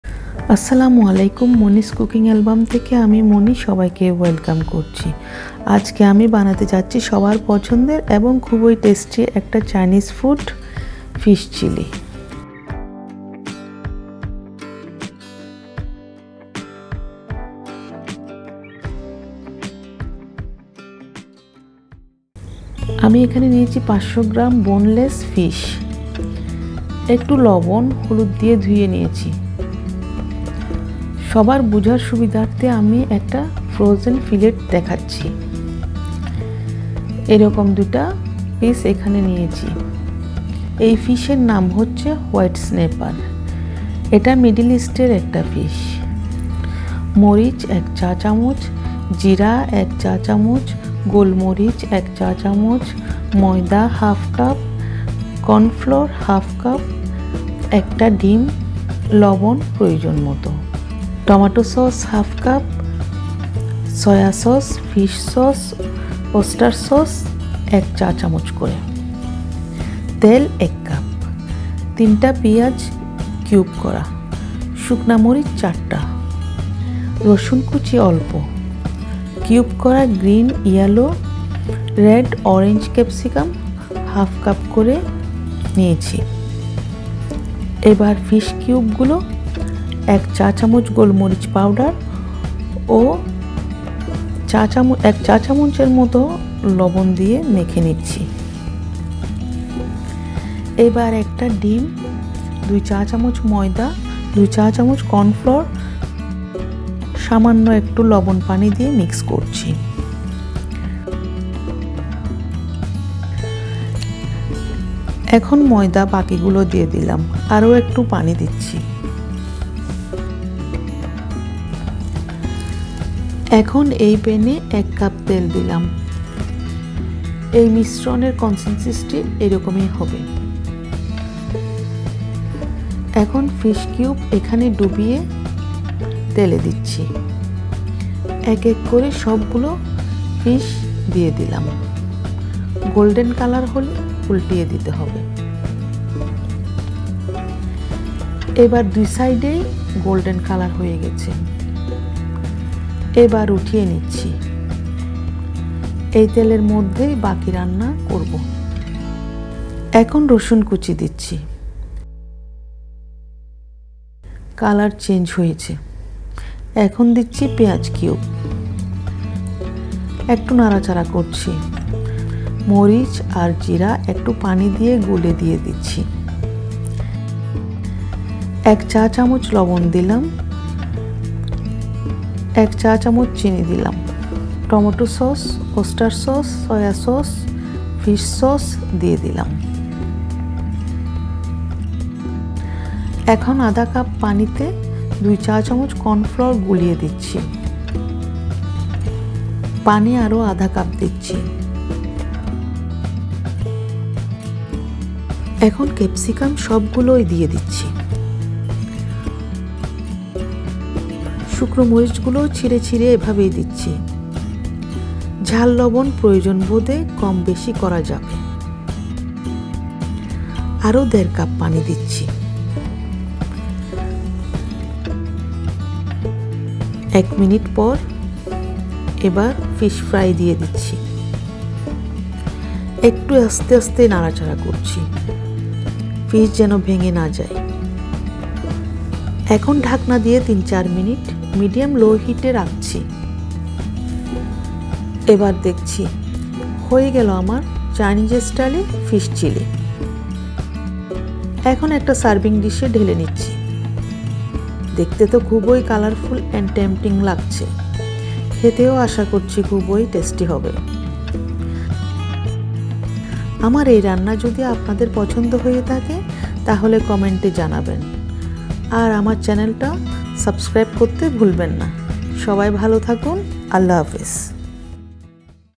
ফিস্ চিলি | Chinese Recipe | Fish Chili | Moni's Cooking Album
*** INGREDIENTS ***
500g Cubed Fish
1 Tea Spoon Chili Powder and Cumin
1 Tea Spoon Black Pepper
Salt as per your requirement
1/2 Cup Tomato Sauce
1 Tea Spoon Fish Sauce, Soya Sauce and Oyster Sauce
1 Cup Oil
3 Chopped Onions
1/2 Cup Capsicum (4 different colors)
1 Egg
Few Garlic Chops
1/2 Cup Flour (Maida)
1/2 Cup Corn Flour

500 গ্রাম মাছ, 1 চা চামচ মরিচ জিরা গোল মরিচ, লবন আধা কাপ, টমেটো সস, সয়া সস ফিস সস ওসটার সস, পেয়াজ, রসুন, কেপসিকাম, তেল, ডিম, ময়দা, কণফলর

*** SHOOTING ACCESSORIES ***
Camera - Samsung Galaxy S9